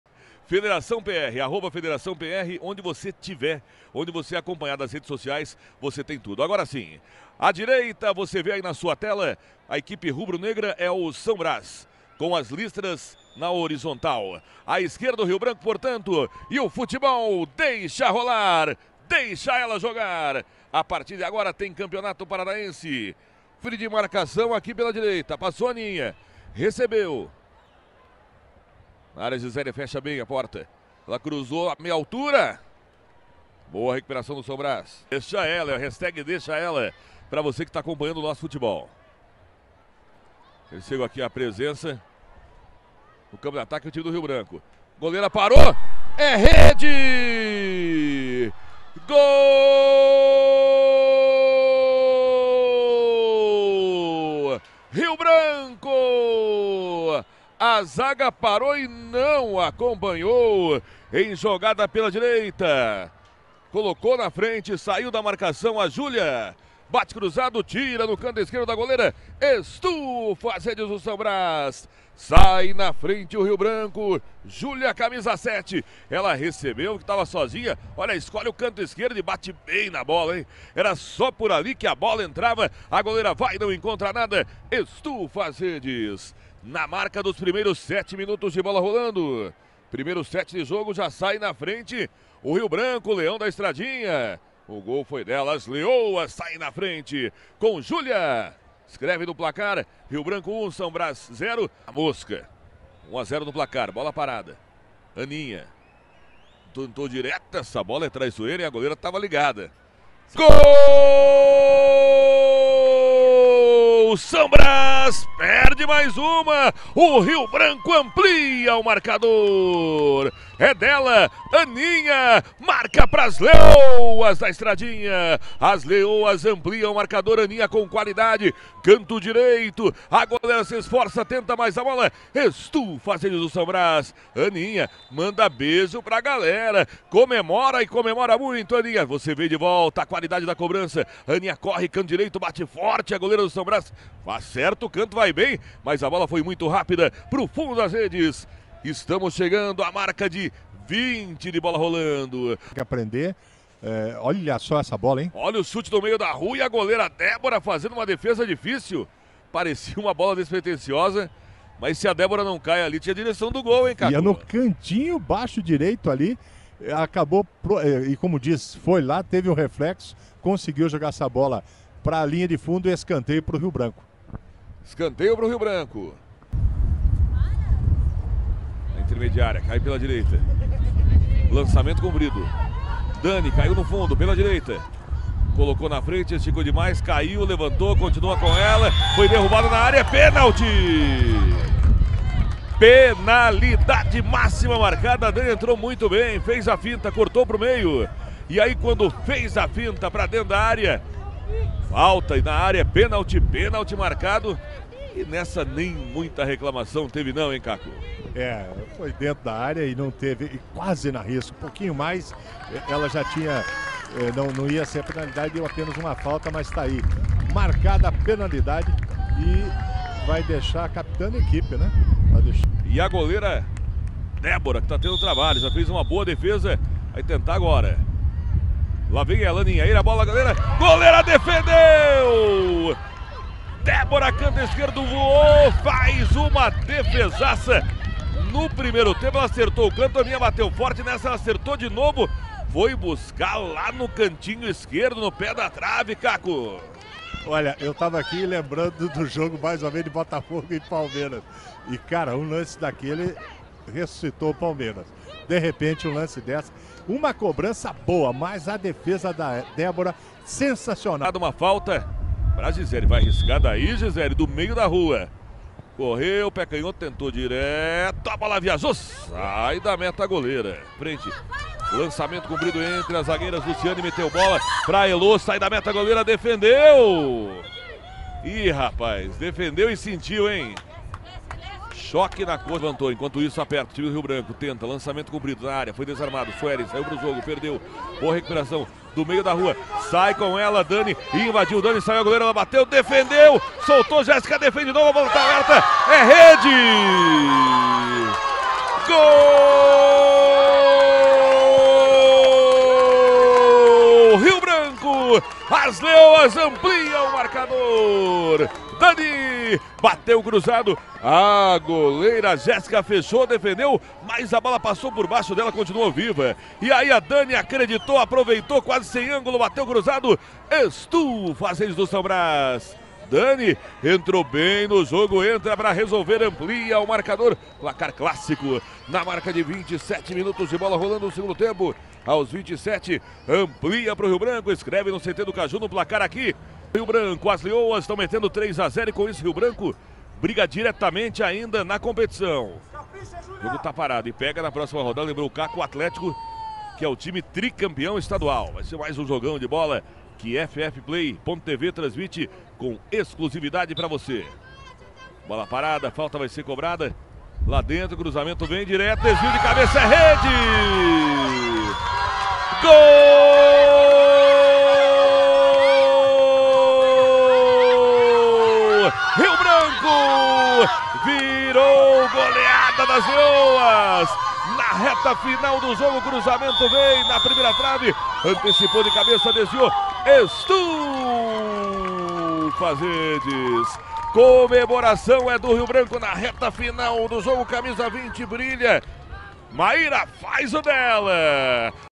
MELHORES MOMENTOS | SÃO BRAZ 0X4 RIO BRANCO | 2ª RODADA DO CAMPEONATO PARANAENSE FEMININO 2023
SÃO BRAZ X RIO BRANCO se enfrentaram pela 2ª rodada da do Campeonato Paranaense Feminino, com transmissão da FPF Play.

Federação Paranaense de Futebol. Levando o nosso esporte até você!